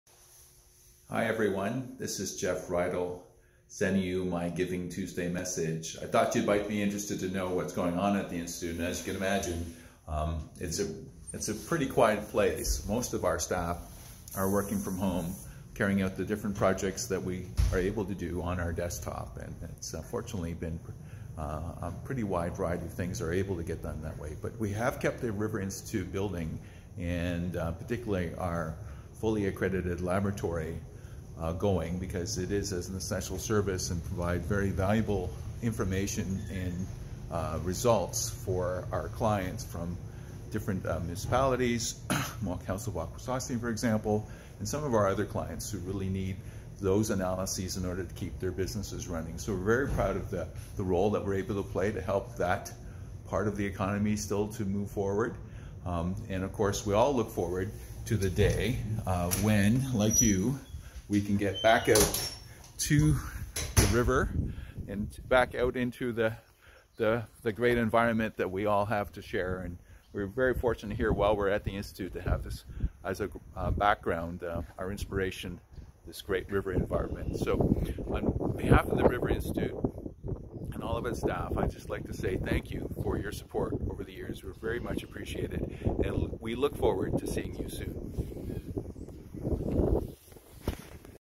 River Institute Tour With Executive Director, Dr. Jeff Ridal
Join Dr. Jeff Ridal on a brief tour of the St. Lawrence River Institute building and a little information about what it's like to be working at the office during the COVID 19 pandemic.